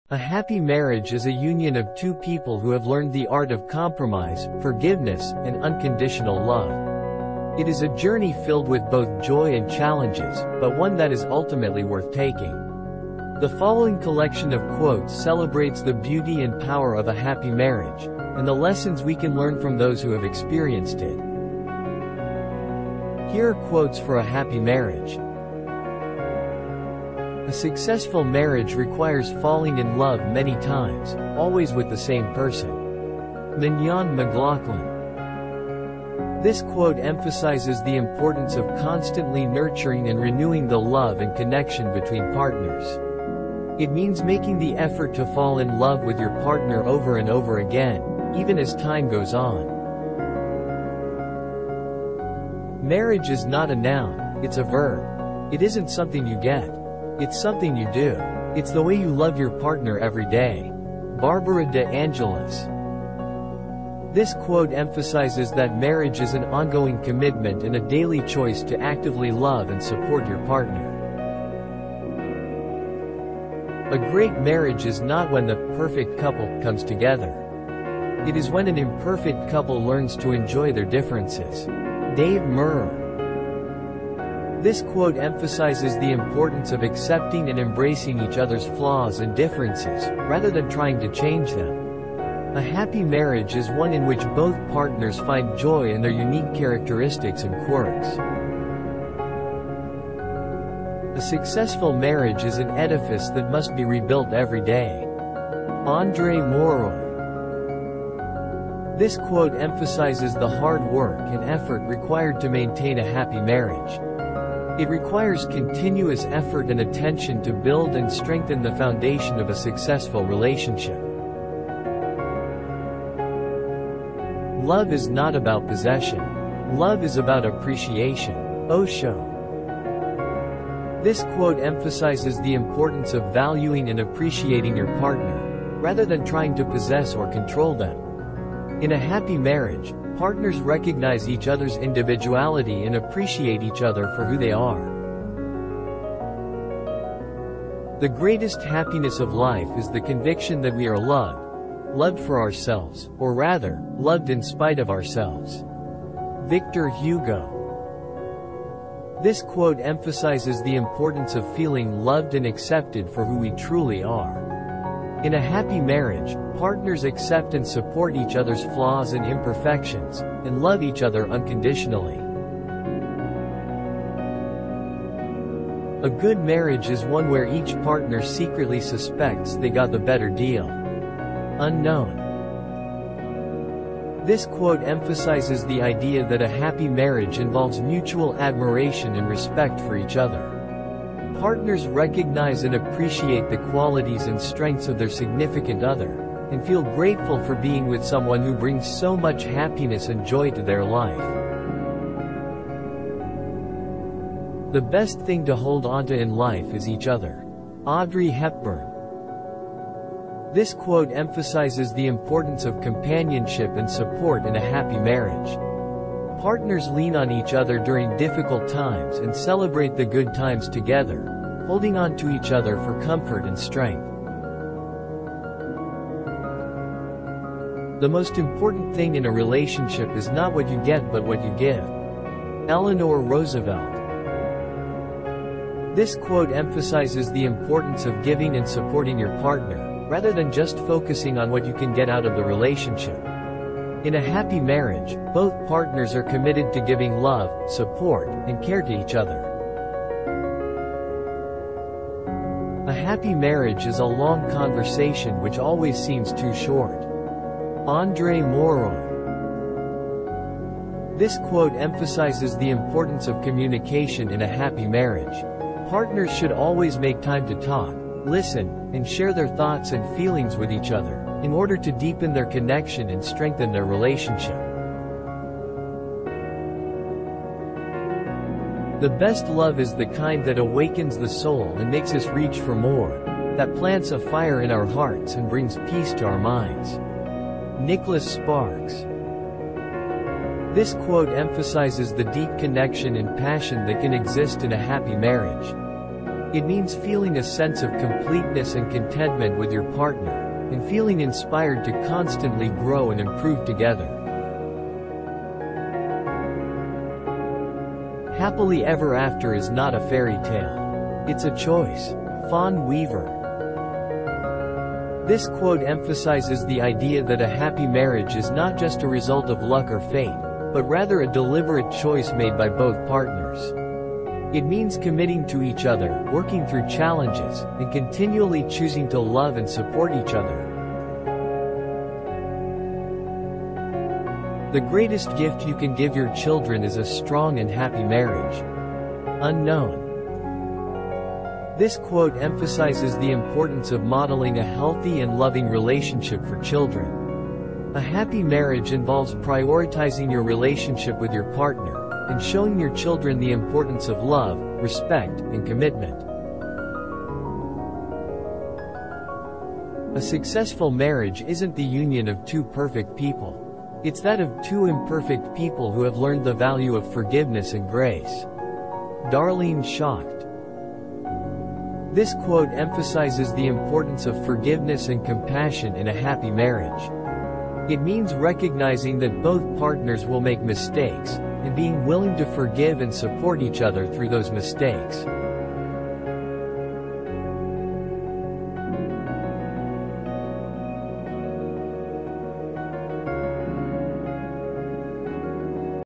"Blissful Bond: Inspiring Quotes on the Art of Happy Marriage"
"Looking to strengthen your relationship and create a happy and fulfilling marriage? Look no further than this video! Filled with inspiring quotes, insightful advice, and practical tips, this video is a must-watch for anyone who wants to build a strong and happy union with their partner. Whether you're a newlywed or have been married for decades, you'll find something valuable in this collection of wisdom and experience.

Join us as we explore the secrets of happy marriages, and learn from some of the most celebrated authors, poets, and thinkers of our time. From Maya Angelou to the Dalai Lama, these voices will inspire you to love more deeply, communicate more effectively, and build a life together that is full of joy and meaning. So why wait? Click play now and start your journey to a happy and fulfilling marriage today!

Here are quotes for a happy marriage:
"A successful marriage requires falling in love many times, always with the same person." - Mignon McLaughlin
"Marriage is not a noun; it's a verb. It isn't something you get. It's something you do. It's the way you love your partner every day." - Barbara De Angelis
"A great marriage is not when the 'perfect couple' comes together. It is when an imperfect couple learns to enjoy their differences." - Dave Meurer
"A successful marriage is an edifice that must be rebuilt every day." - Andre Maurois
"Love is not about possession. Love is about appreciation." - Osho
"The greatest happiness of life is the conviction that we are loved; loved for ourselves, or rather, loved in spite of ourselves." - Victor Hugo
"A good marriage is one where each partner secretly suspects they got the better deal." - Unknown
"The best thing to hold onto in life is each other." - Audrey Hepburn
"The most important thing in a relationship is not what you get but what you give." - Eleanor Roosevelt
"A happy marriage is a long conversation which always seems too short." - Andre Maurois
"The best love is the kind that awakens the soul and makes us reach for more, that plants a fire in our hearts and brings peace to our minds." - Nicholas Sparks
"Happily ever after is not a fairy tale. It's a choice." - Fawn Weaver
"The greatest gift you can give your children is a strong and happy marriage." - Unknown
"A successful marriage isn't the union of two perfect people. It's that of two imperfect people who have learned the value of forgiveness and grace." - Darlene Schacht